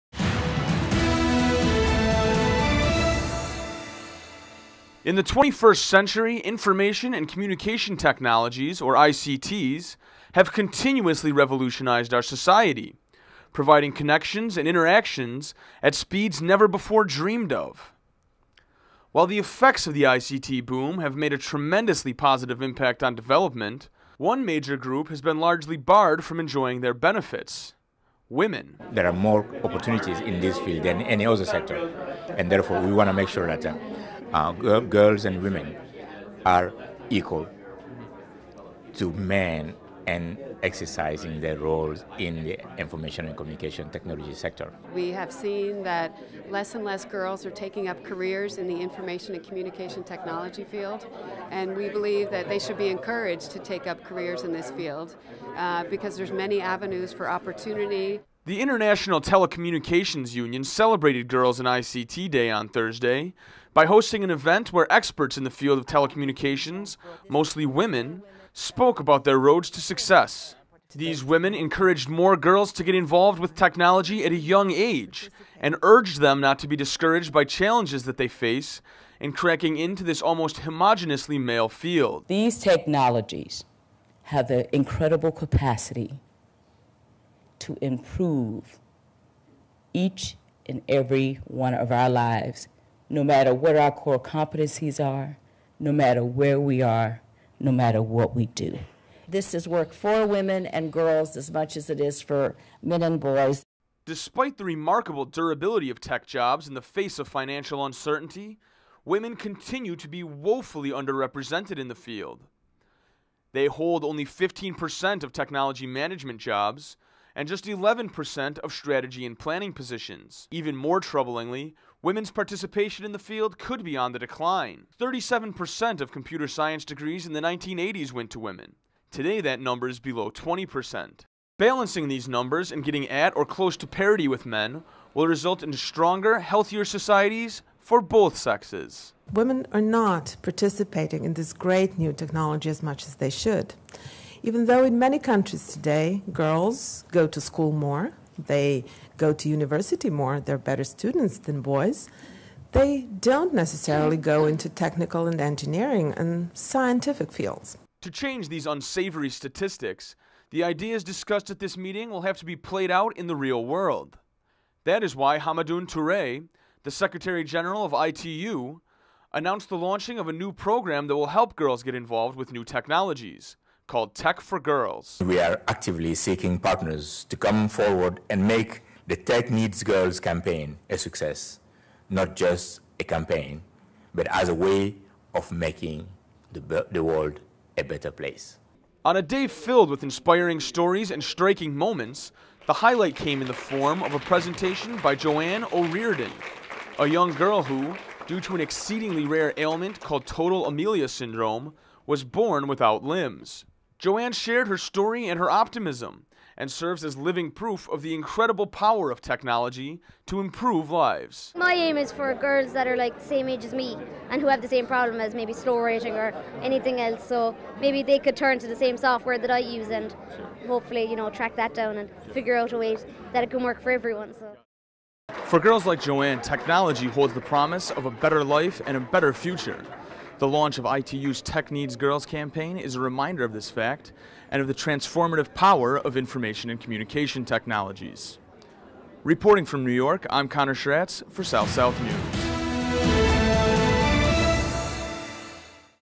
A Special report on ITU's Girls in ICT Day in New York by South-South News
For a complete video of Joanne O'Riordan's keynote speech go to the playlist "Girls in ICT" on the ITU YouTube channel's main